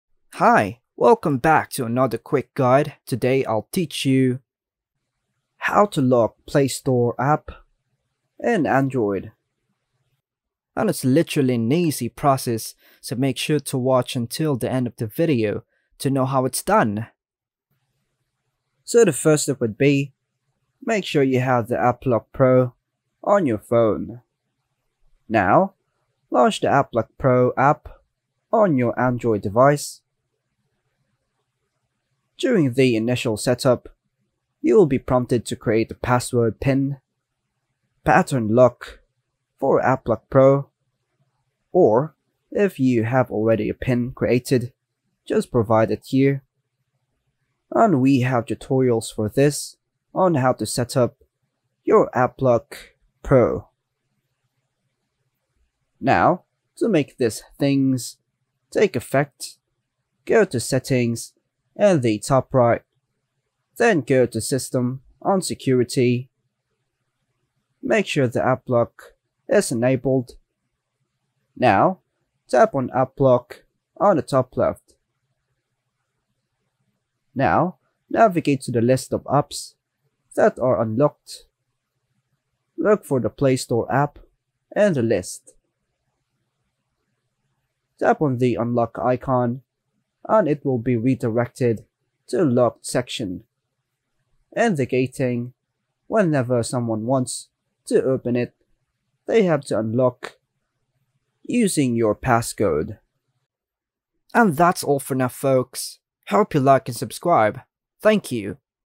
How To Lock Play Store App In Android (Step-by-Step Guide)
This guide will show you how to lock Play Store app in Android. This is especially helpful if other people (kids) have access to your phone so that you can prevent unwanted downloads and purchases on the Android Playstore.